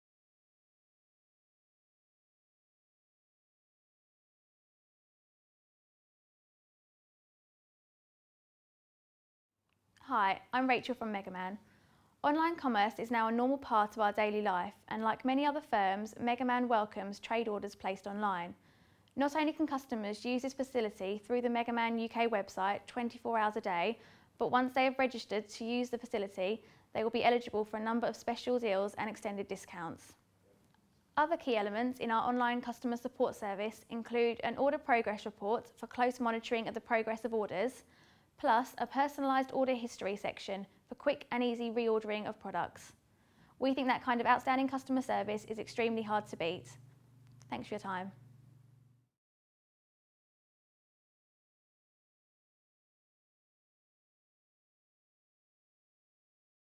Megaman Online Ordering Service
Megaman now offer an online ordering service to their wholesale customers.